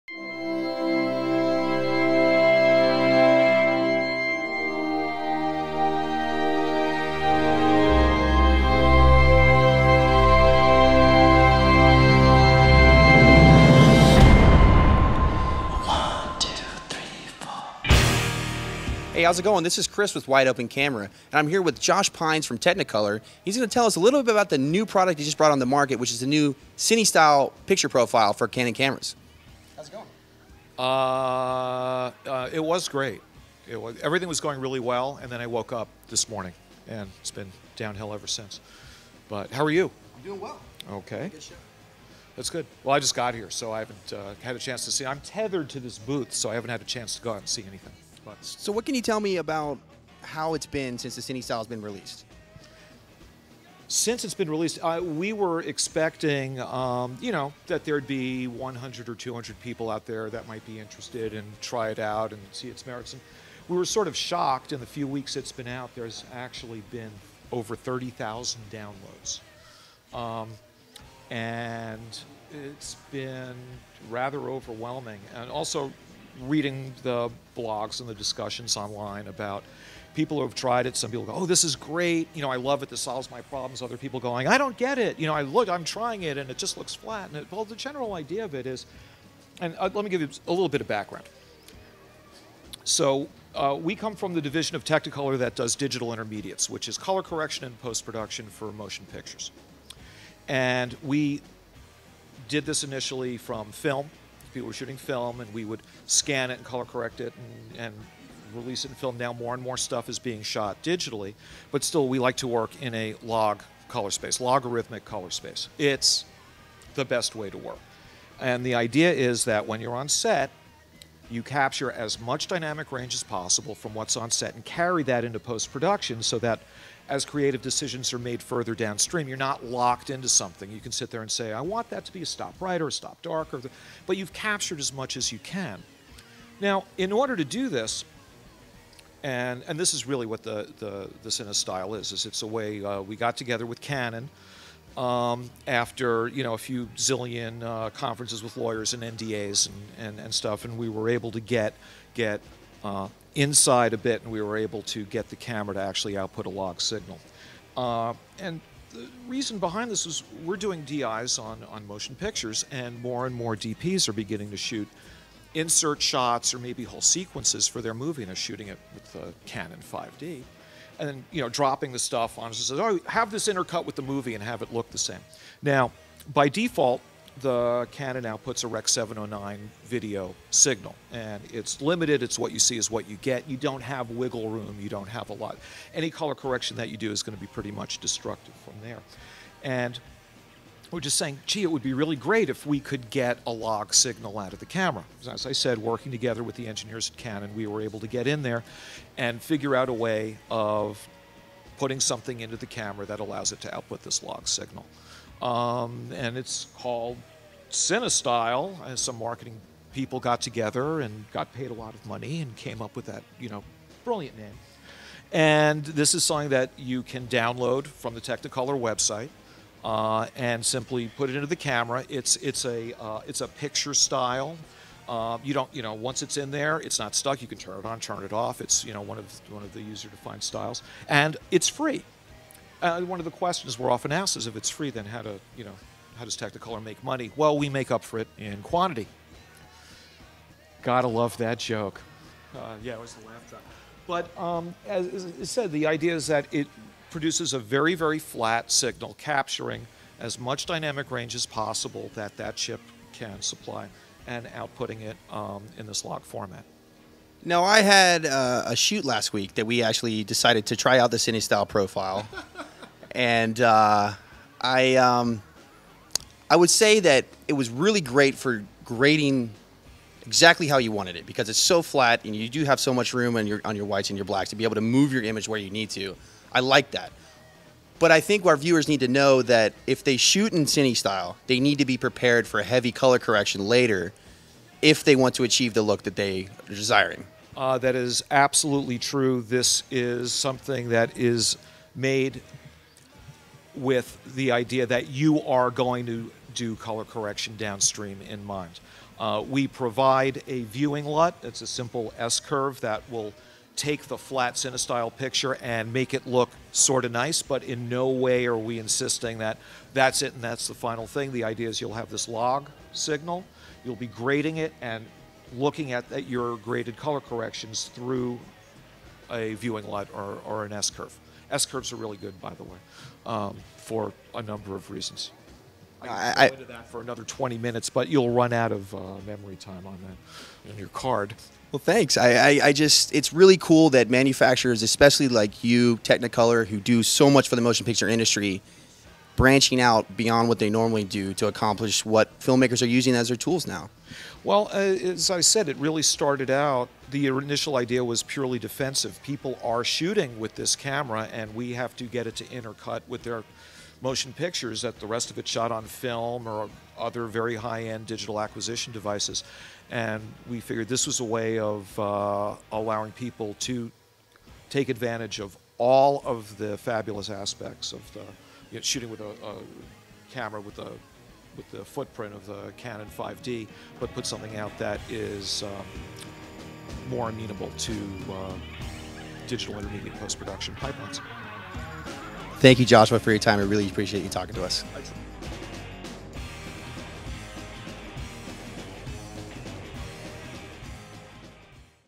Cine Gear 2011 Josh Pines of Technicolor
Chris Marino talks with Josh Pines of Technicolor about the recently released Canon DSLR's Picture Style profile released by Technicolor.